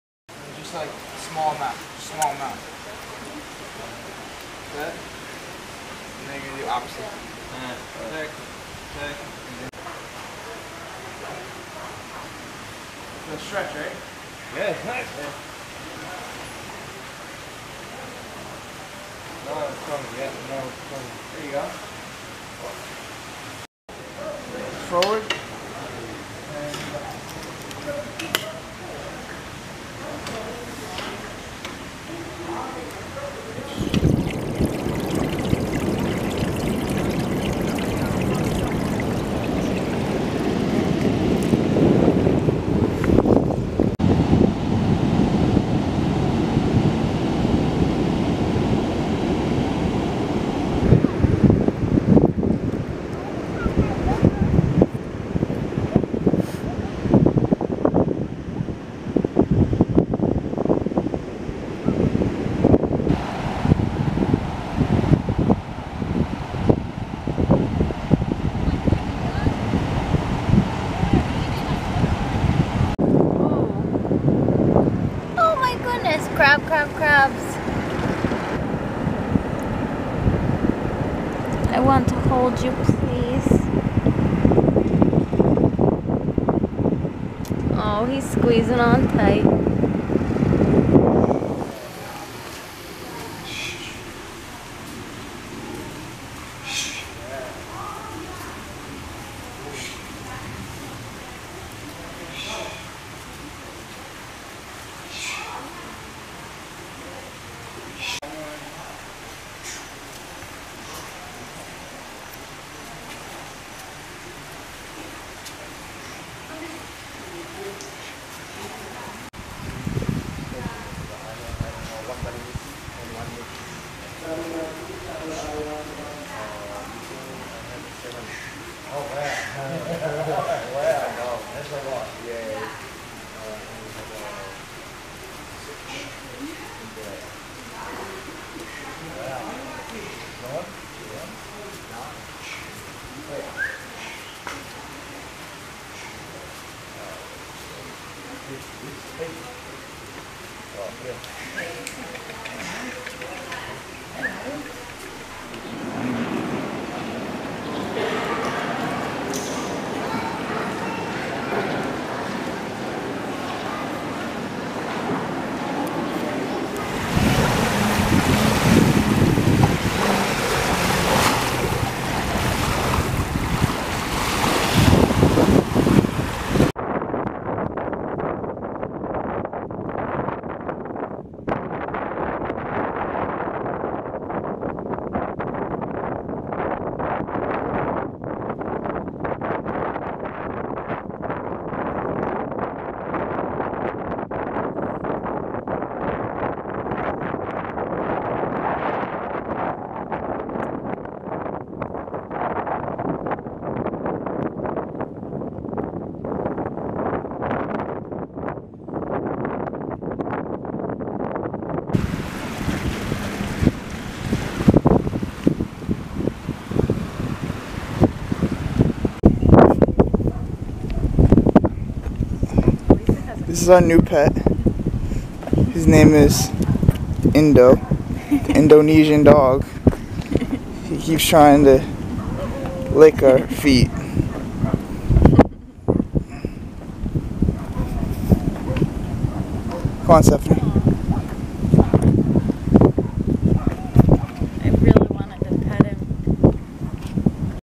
Bali Honeymoon: Sanur Part 2
-Animal flow
-Indian Ocean
-Melasti Beach
-Capoeira
-Catholic church
-Stray dog